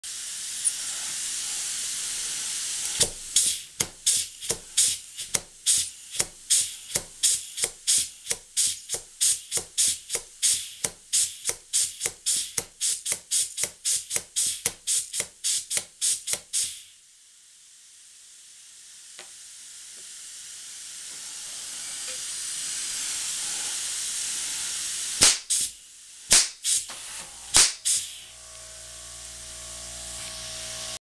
MVI 2977 Steam Hammer
First strikes from newly built model steam hammer, based on the Stuart Turner design. The column is their casting, rest is from bar stock.